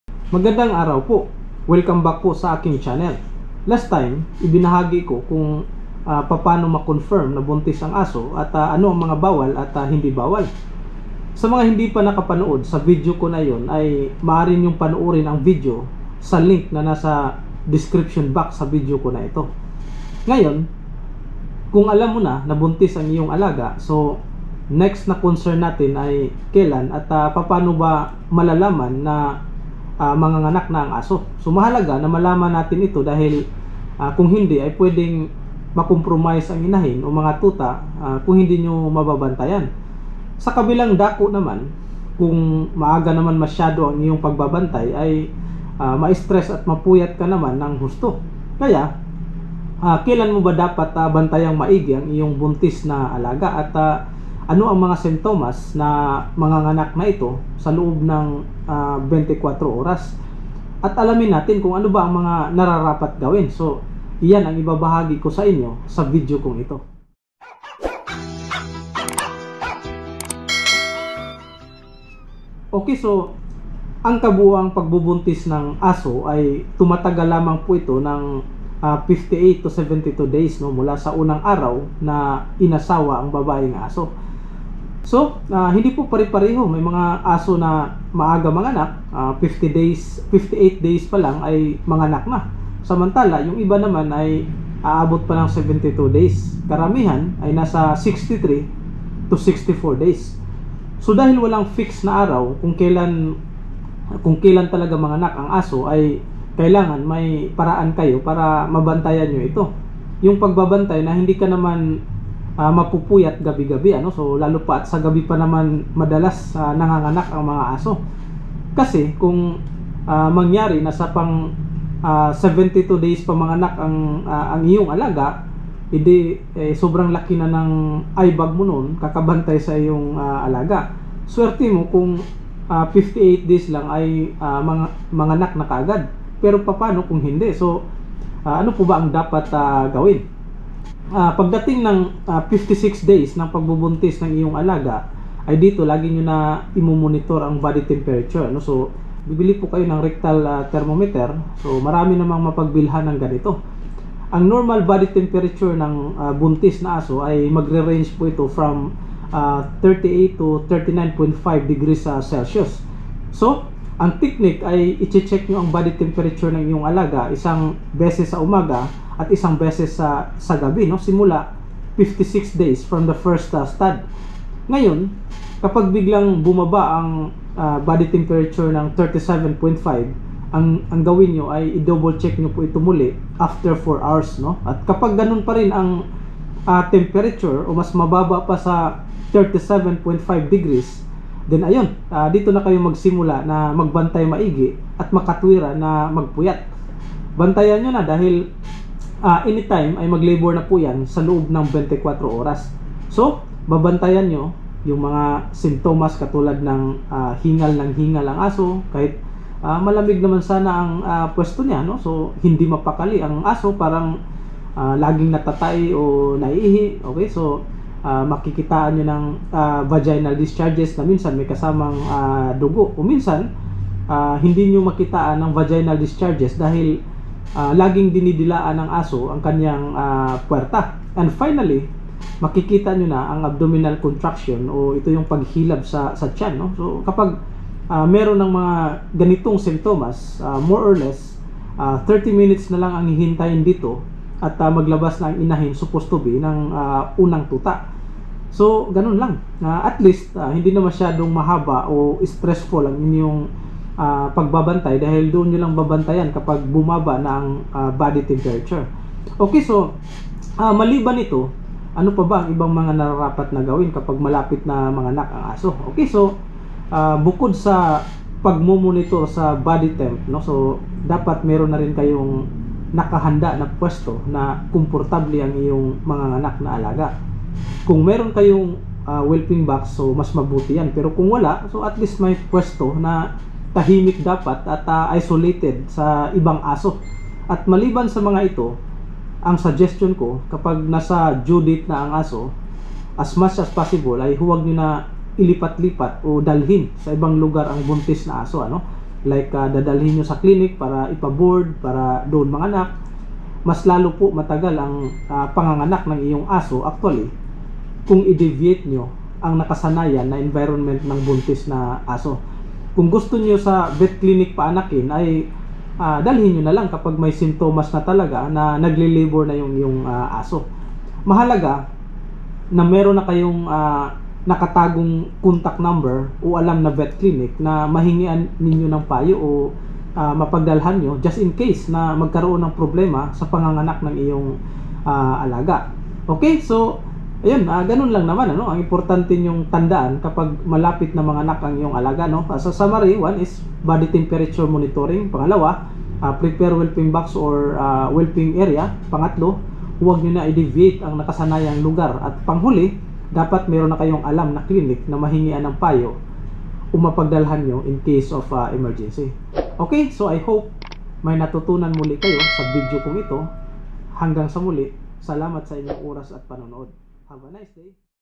MGA DAPAT GAWIN KAPAG MALAPIT NA MANGANAK ANG ASO | Paano Malaman Na Manganak Na Sa Loob Ng 24 oras?
Ang video na ito ay magbigay kasagotan sa mga pet owner na nagtatanong tungkol sa kanilang buntis na aso kung ano ba ang mga nararapat gawin kapag malapit na manganak?Ano ang mga simtomas na manganak na ito sa loob ng 24 oras ng sa ganun ay mababantayang maigi ang panganganak ng kanilang alaga at ng maiwasan ang problema sa inahin o sa mga tuta.